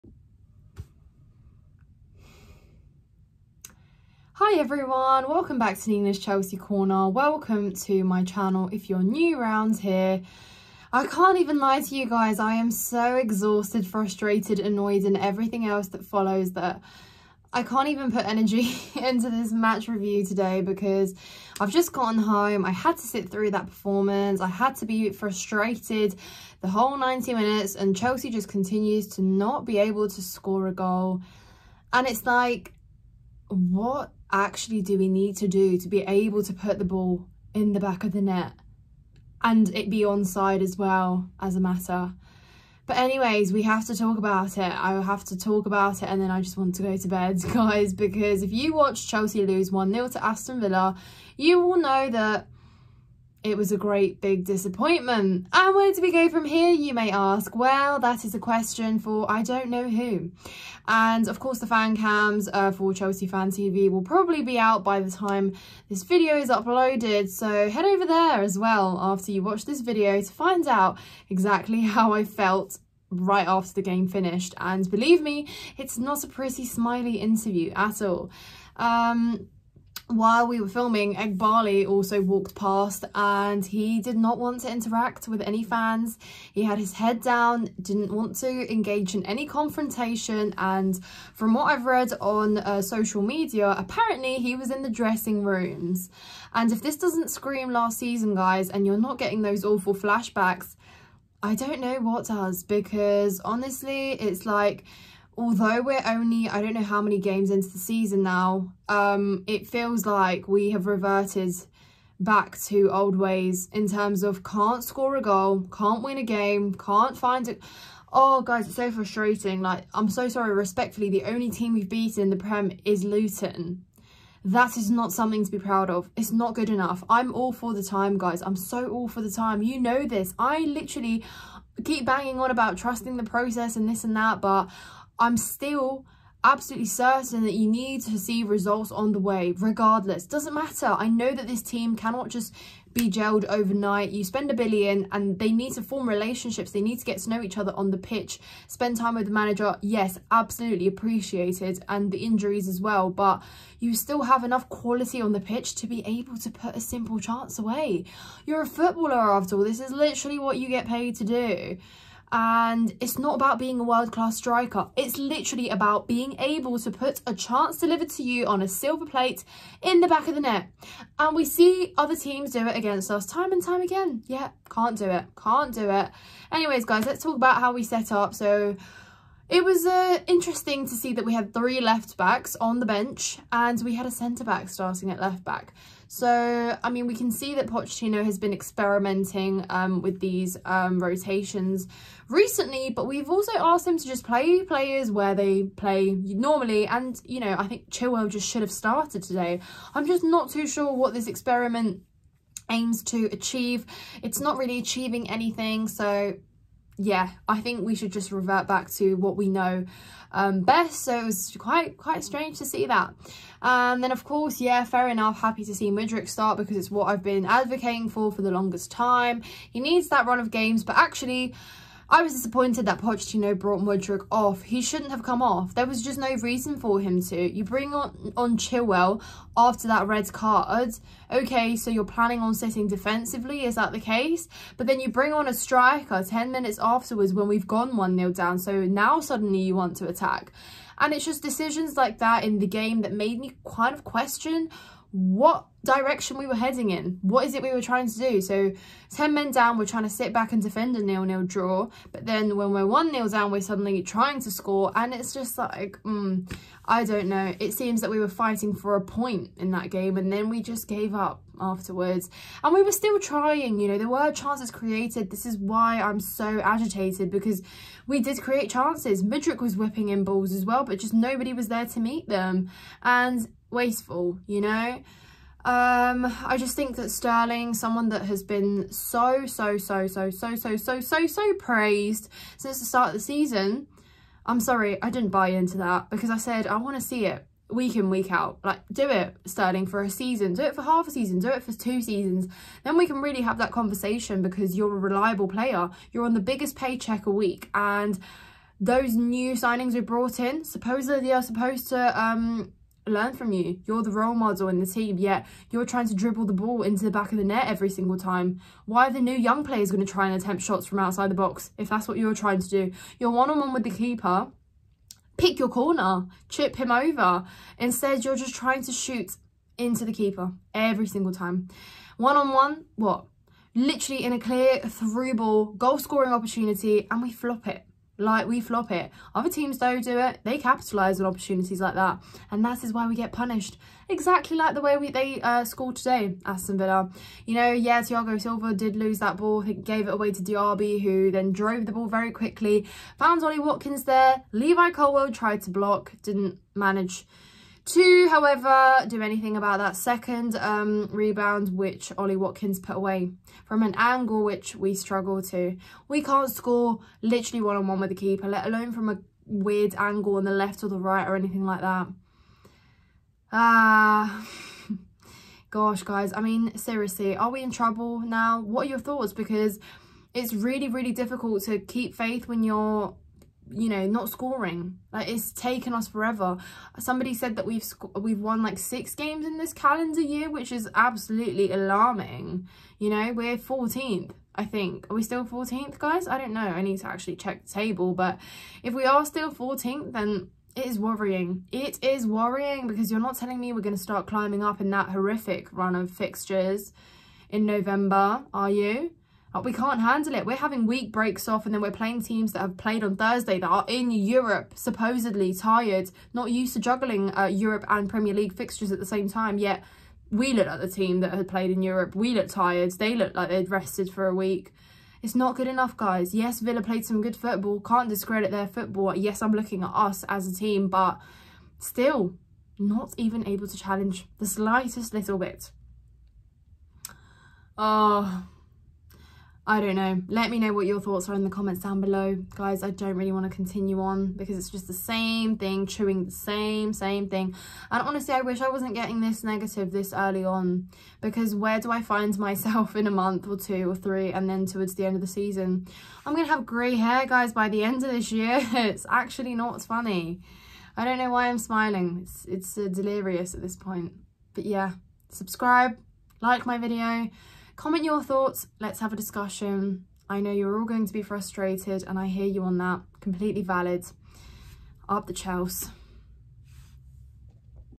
Is Our Season In Trouble? | Chelsea 0 - 1 Aston Villa | Match Review
*sigh* it’s a defeat at home against Aston Villa.

What is going wrong? Not a good enough start to the season…